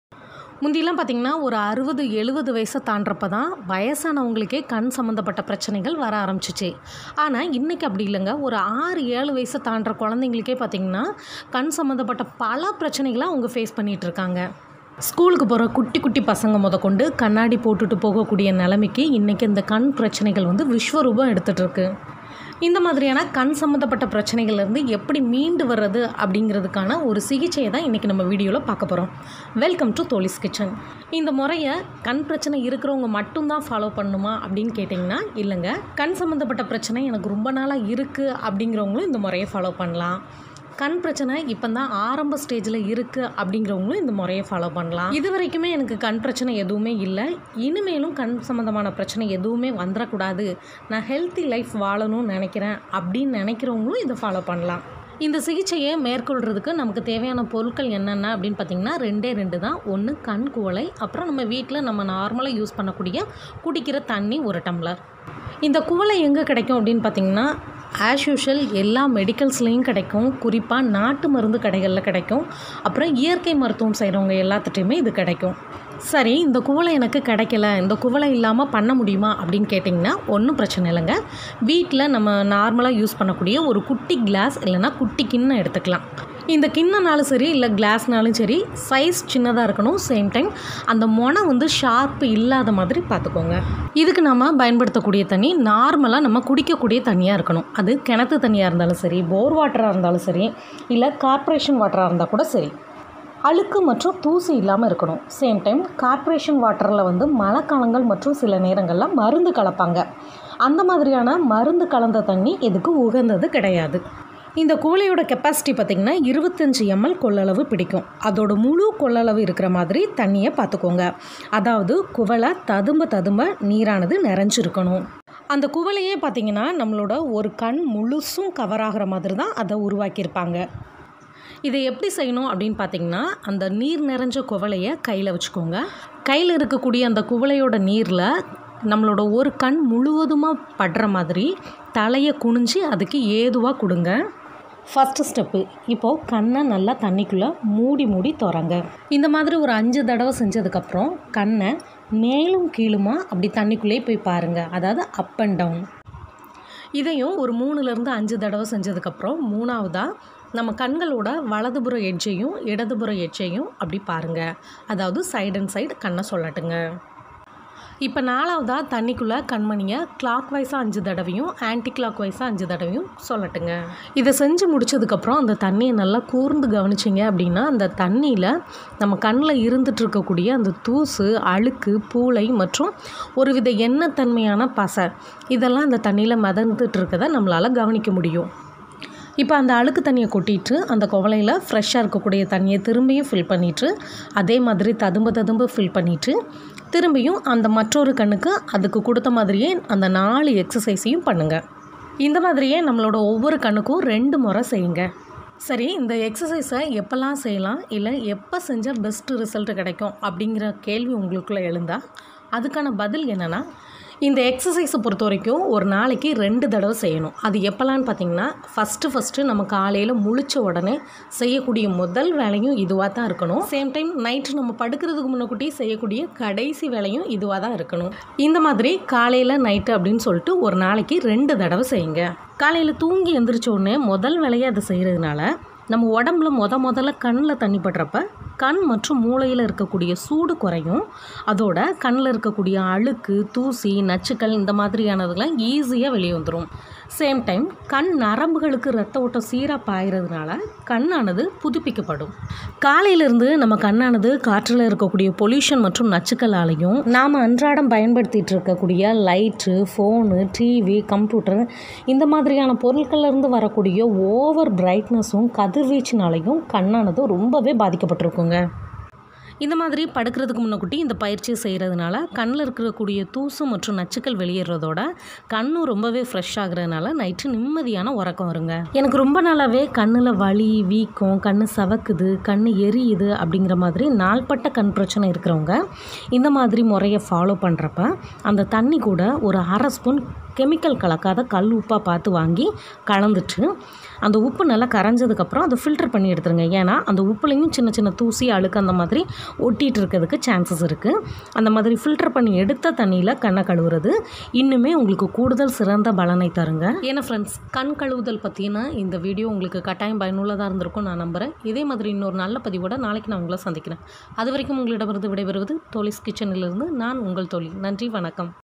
கண் கழுவும் பயிற்சி செய்வது பற்றிய முழுவிளக்கத்துடன் | Eye wash method full details
நாம் வாழ்ந்து கொண்டிருக்கும் இந்த காலகட்டத்தில் நம்மை சுற்றி சூழ்ந்து இருக்கும் காற்று தூசு மற்றும் மாசு நிறைந்தது.

அத்தகைய மாசு தூசுகளிலிருந்து நம் கண்களை பாதுகாக்க மற்றும் கண் சம்பந்தமான அனைத்து பிரச்சினைகளிலிருந்தும் கண்களை பாதுகாப்பதற்கான எளிய வழி